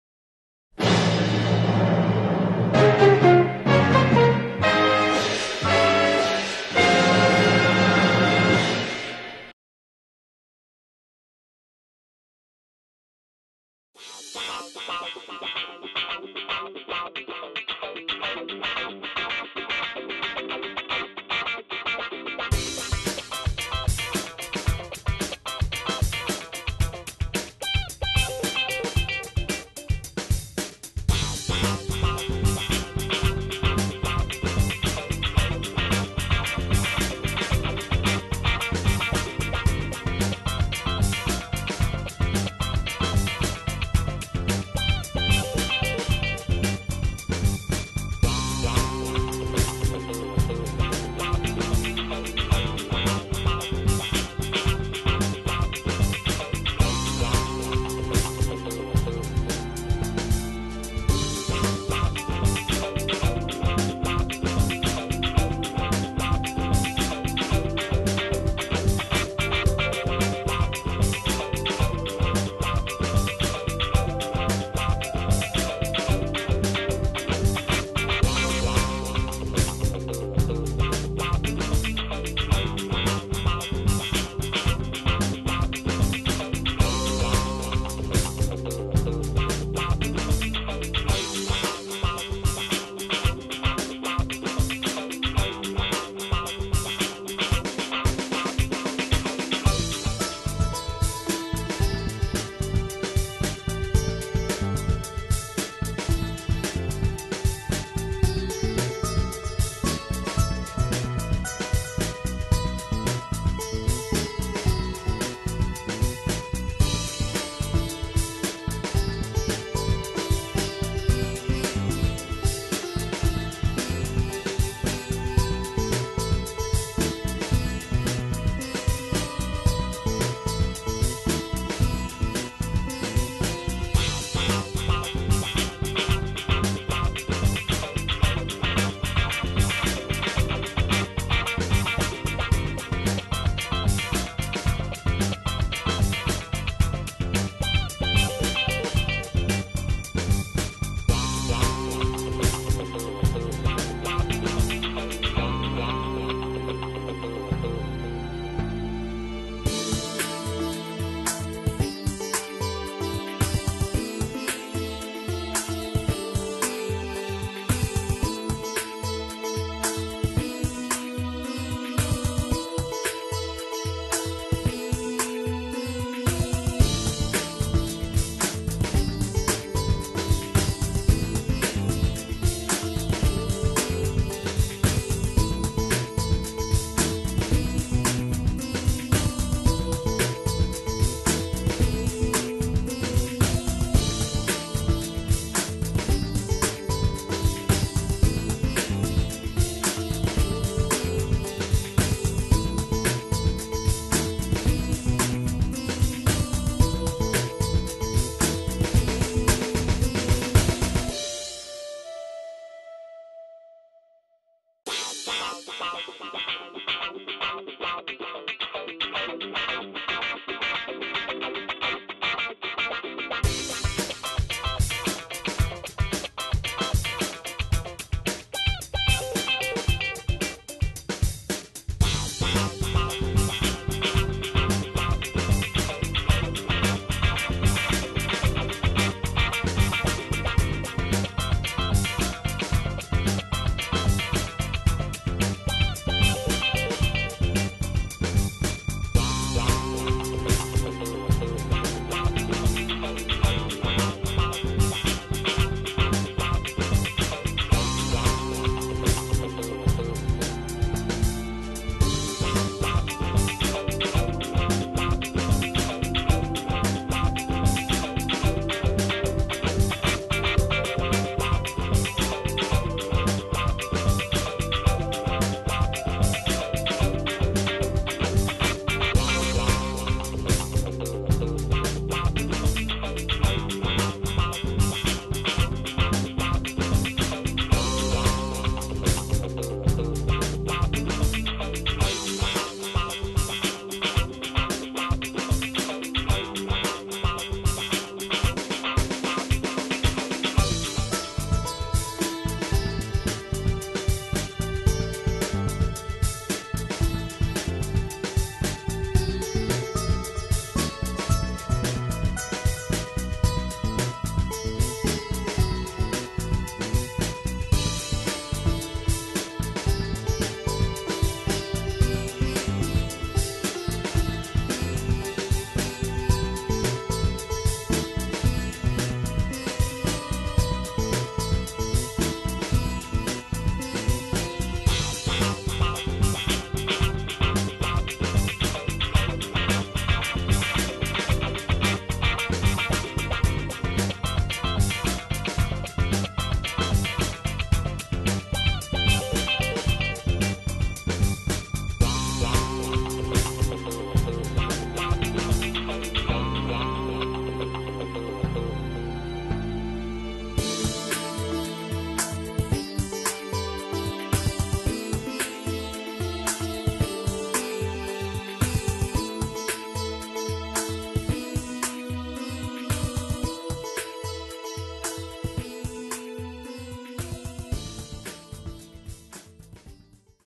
Izumi Matsumoto in Action (left hand) - mastersofmanga.com
Izumi Matsumoto draws Hikaru Hiyama in front of a camera and with his left hand! (he is right-handed).